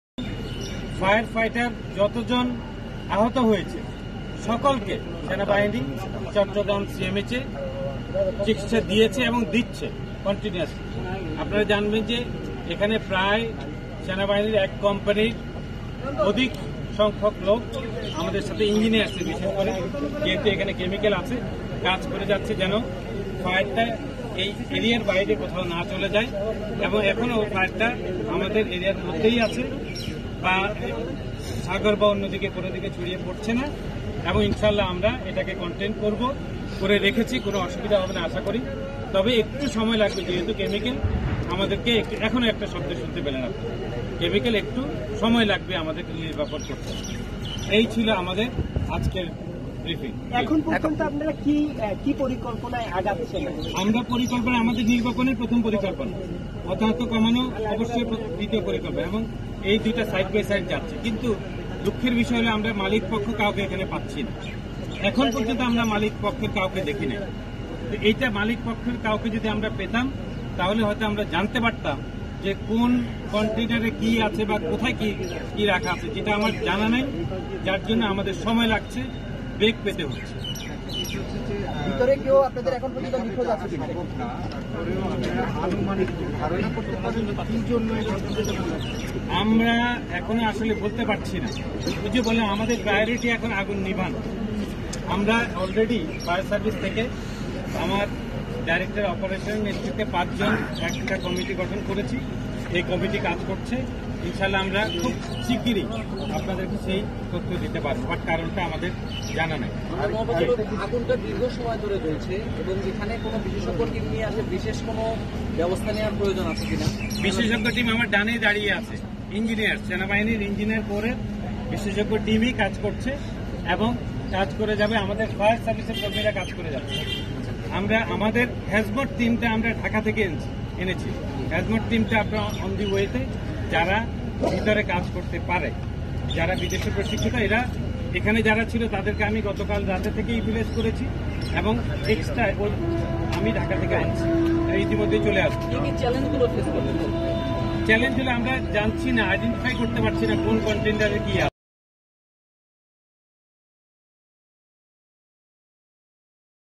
‌‘মালিক পক্ষের কাউকে খুঁজে পাওয়া যাচ্ছে না’ | CTG Fire | Fire Service | Desh tv news
‌‘মালিক পক্ষের কাউকে খুঁজে পাওয়া যাচ্ছে না’ |

»» Office Address:
42, Shaheed Sangbadik Selina Parveen Sarak, Malibag 1217 Dhaka, Dhaka Division, Bangladesh